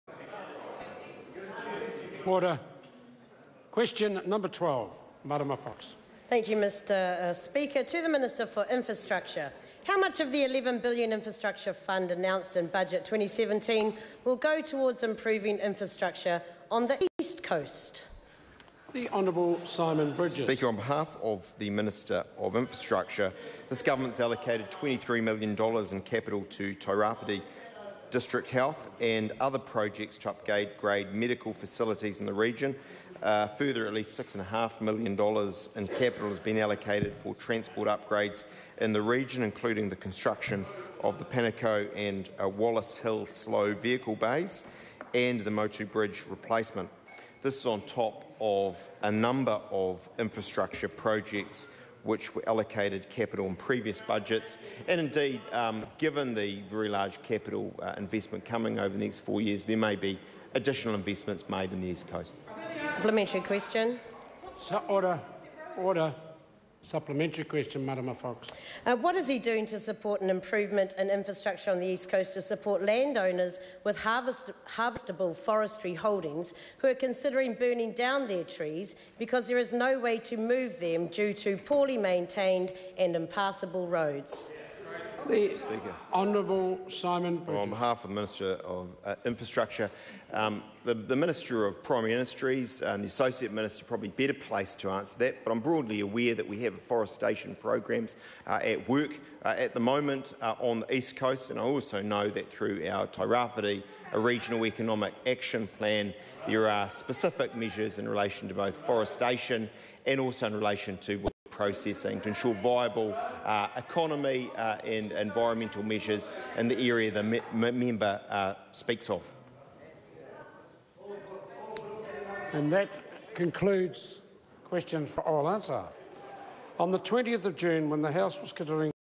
Question 12 - Marama Fox to the Minister for Infrastructure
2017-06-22 - Question 12 - Marama Fox to the Minister for Infrastructure

Simon Bridges,Marama Fox